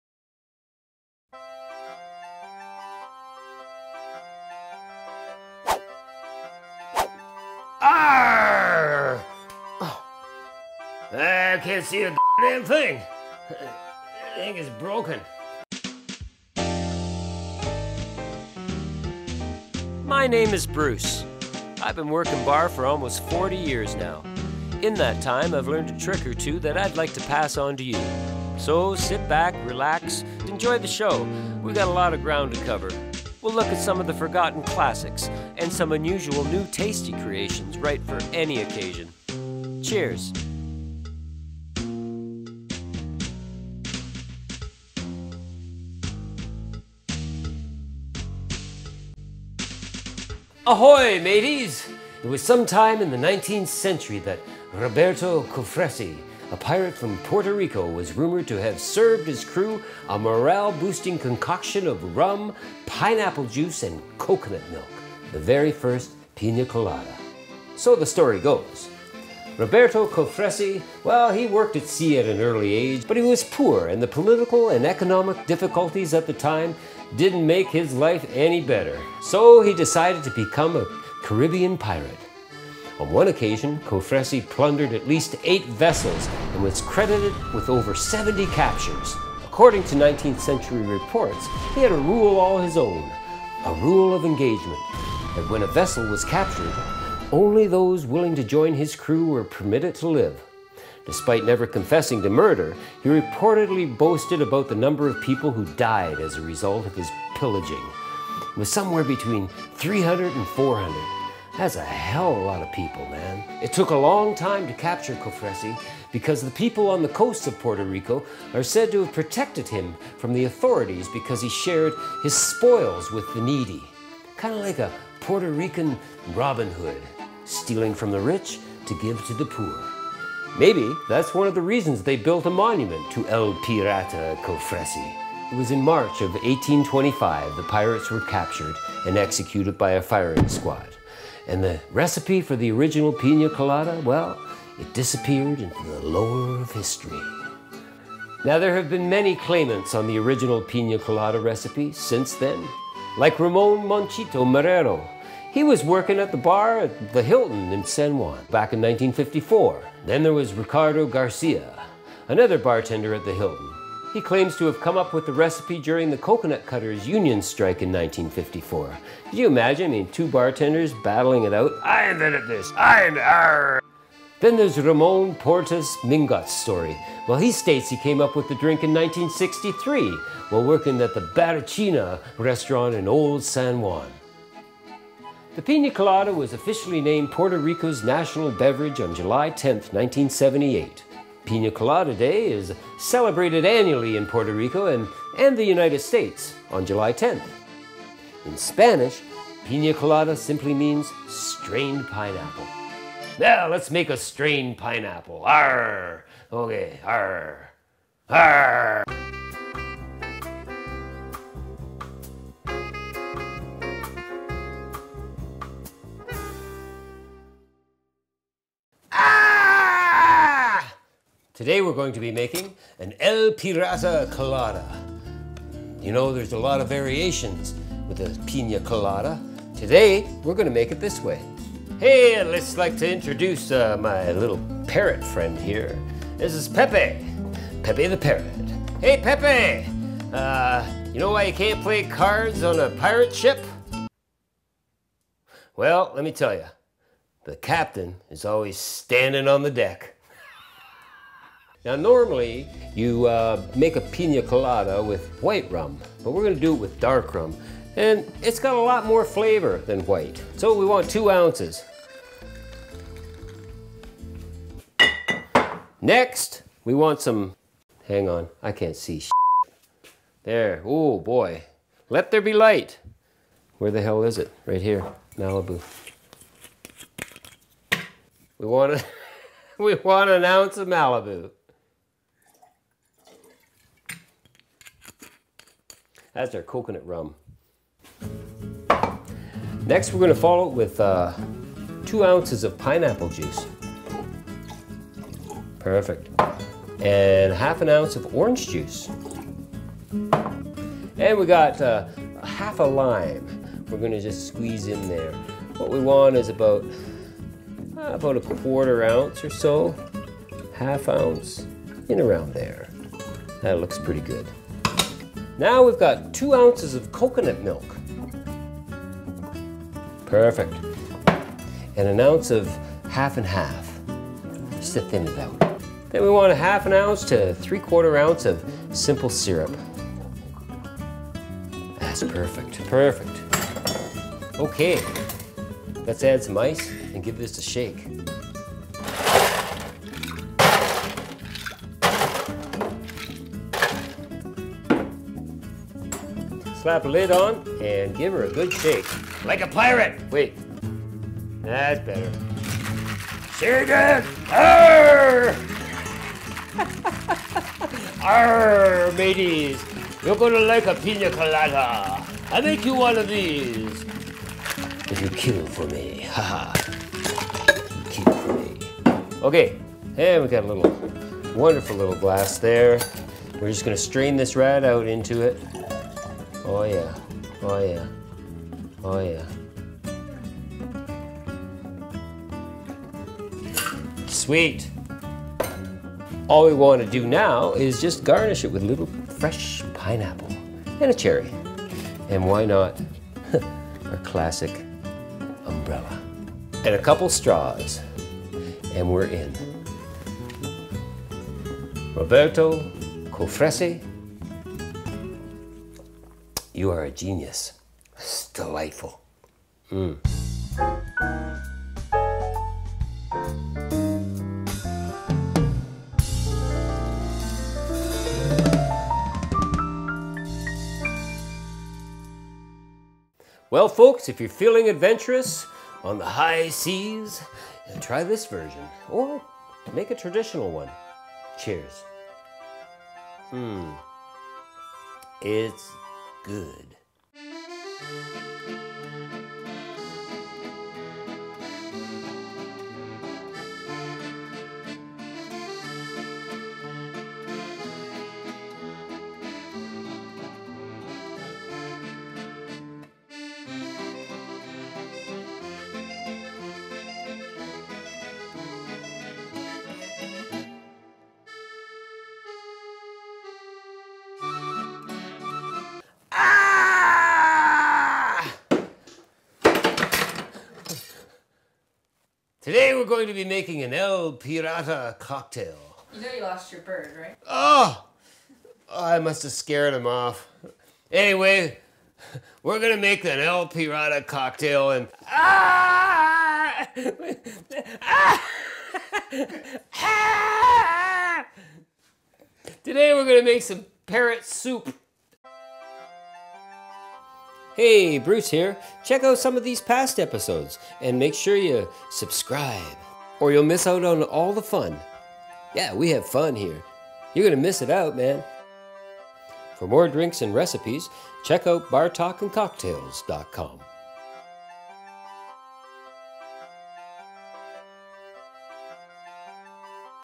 Piña Colada! - El Pirata Colada and Pirate Roberto Cofresi | BAR TALK & COCKTAILS
Arrrr! The Piña Colada has a debated history. If you go back far enough you will find it's roots lie with a 19th century Puerto Rican Pirate. Thought we'd create a new version in honour of his name.
El Pirata Cofresi.

We go a little off the rails on this one.

Credits
Original music written and preformed by Bruce
Select Stock Footage provided by Videoblocks.com
Select images provided by wikimedia.

Video production and post production by Rev Media Group